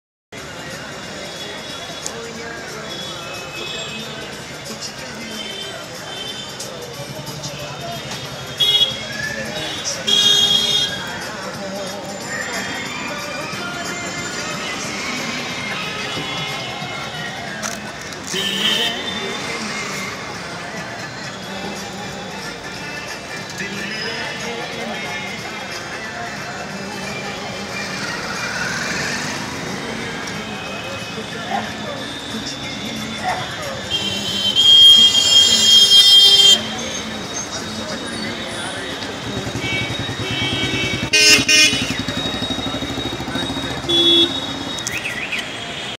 Jain mandir chandni chowk delhi.
Shri Digambar Jain Lal Mandir is the oldest and best-known Jain temple in Delhi, India. It is directly across from the Red Fort in the historical Chandni Chowk ...